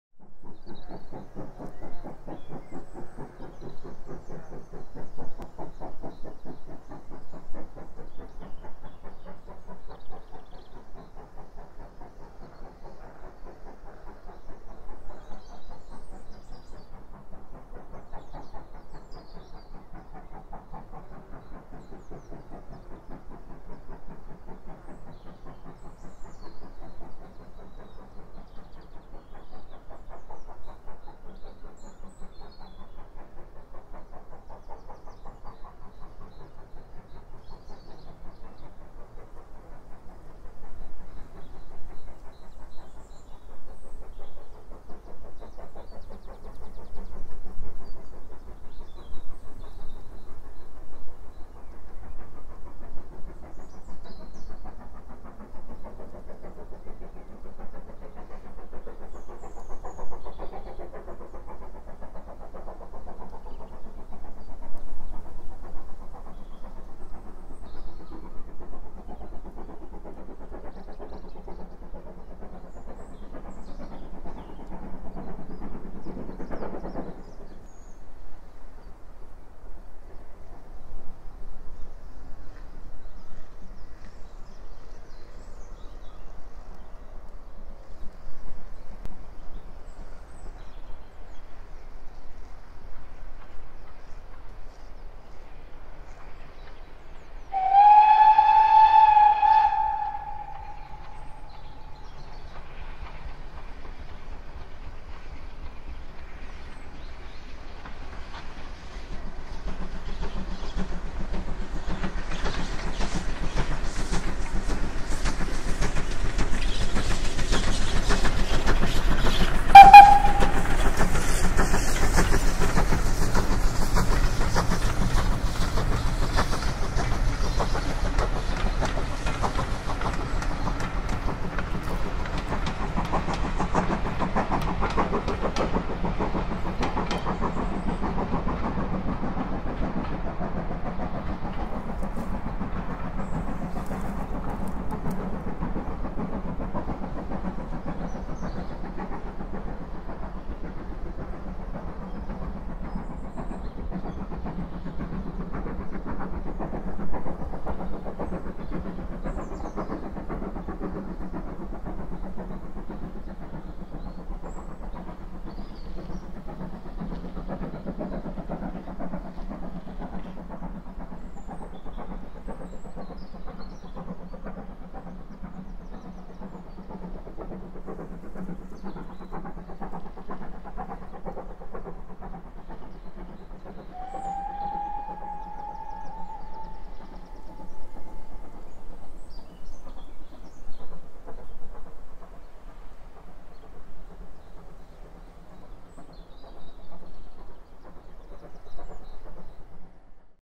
West Somerset railway 10 April 2022 Watchet Mineral Line GWR 9351
9351 seen here on the outskirts of Watchet and following the Mineral Line which ends up at Washford. Sound is original recording as it happened. The Drone does not have any Audio capabilities, so the Sound you hear is recorded using a Tascam DR-05 Field recorder and added to the Final Video Edit ...as best I could!